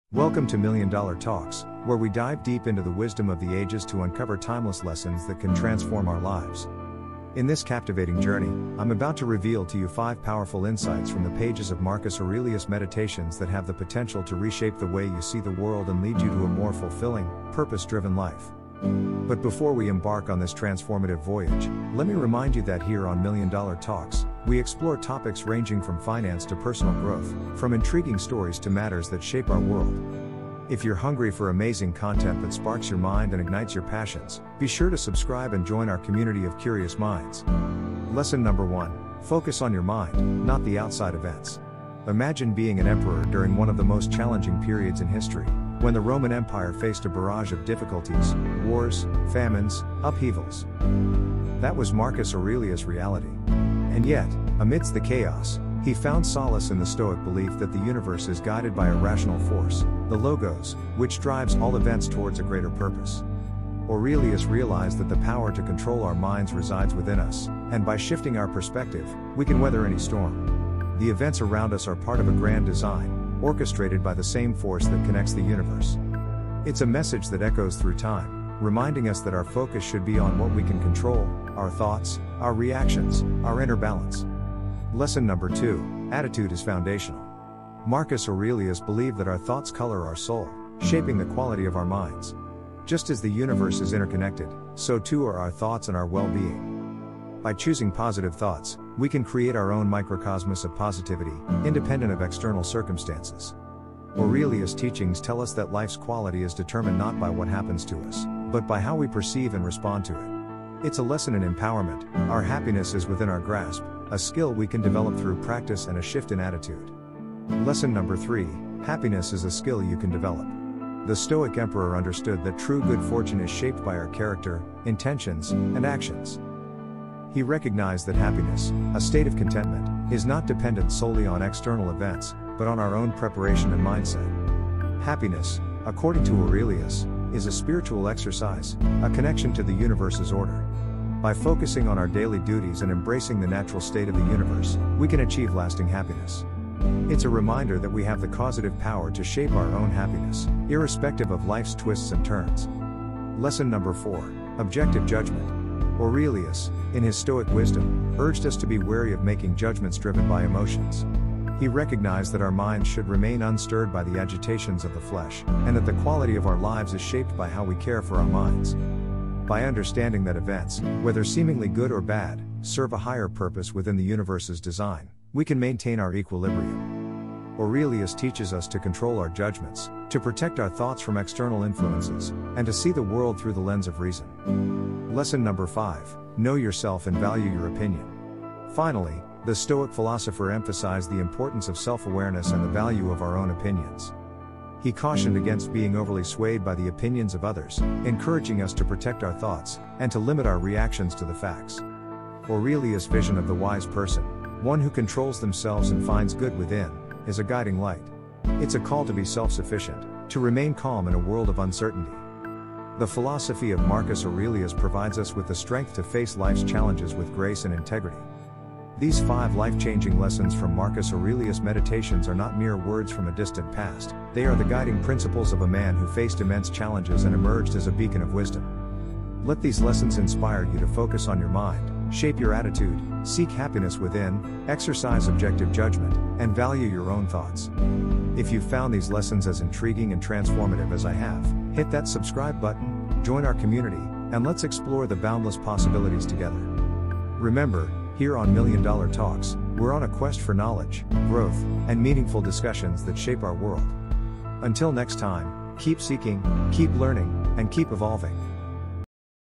5 Life Changing Lessons from Marcus Aurelius Book! (Short Documentary)
Discover 5 Life-Changing Lessons from Marcus Aurelius' "Meditations" on our channel, "Million Dollar Talks"! 📚

Marcus Aurelius, the Stoic Roman Emperor, faced unprecedented challenges during his reign in the 2nd century AD. Wars, famines, and personal tragedies were his backdrop, and from these trials, he crafted his profound insights on life, happiness, and the power of the mind.

In this video, I'll share with you Marcus Aurelius' teachings, and how they can be applied to our modern lives. We'll explore the importance of focusing on your mind, cultivating the right attitude, embracing happiness as a skill, practicing objective judgment, and valuing your own opinions.

As we delve into these life-changing lessons, you'll gain the tools to navigate challenges, find inner balance, and create a more meaningful existence. These insights, rooted in the wisdom of a great philosopher and leader, are as relevant today as they were centuries ago.

Don't miss out on this empowering and transformative content!